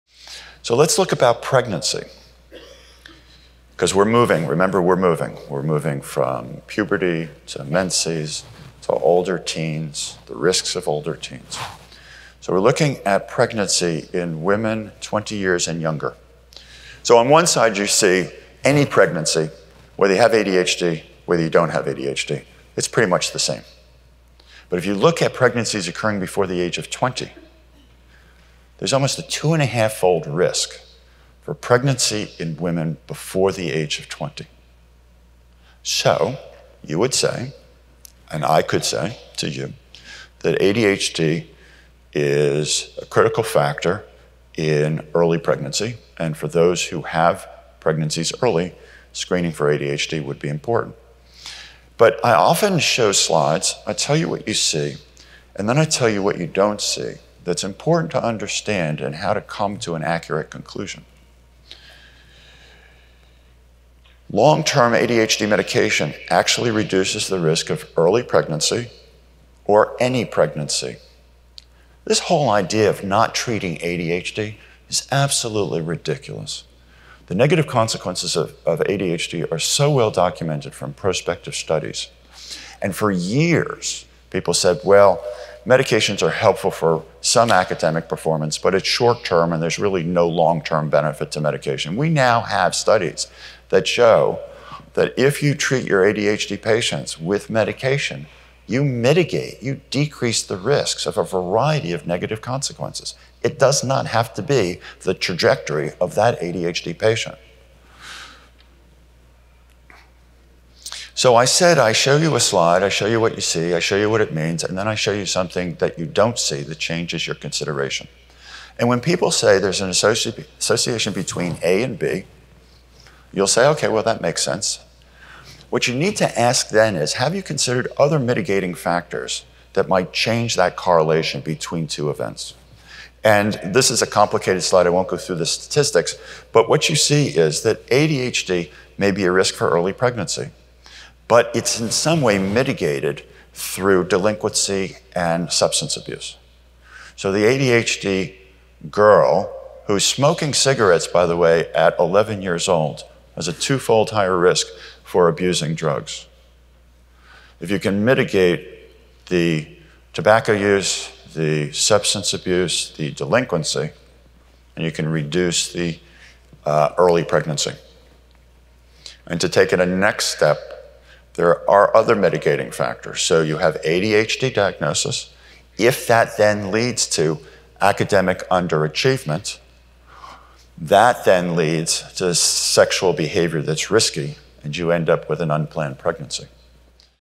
ADHD & Early Pregnancy
Is ADHD a risk factor for early pregnancy? In this presentation, Dr. David Goodman discusses the mitigating factors between ADHD and unplanned pregnancy. Originally presented at the 2022 NEI Synapse. NEI Members can watch the full CME/CE lecture titled, "ADHD, Female Hormones, and Menopause: The Little We Know From the Research"